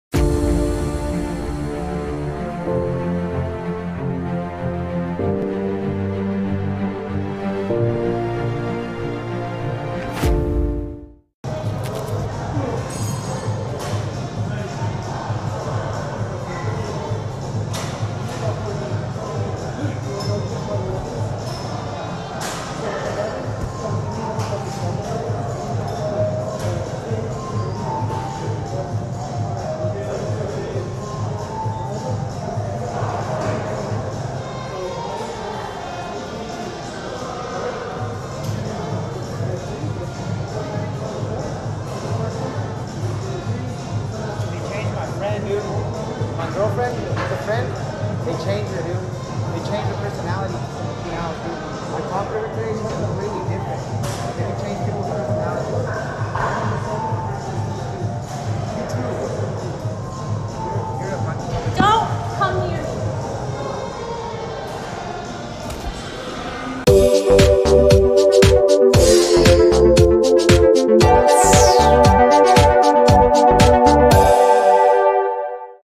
Strange Gym Guy Caught on Camera
Occurred on October 26, 2021 / Midland, Texas, USA: "A male stranger had been bothering me for a few months in the gym, in this video, I happened to be recording my workout when he came up to talk nonsense, then got upset when I ignored him and got just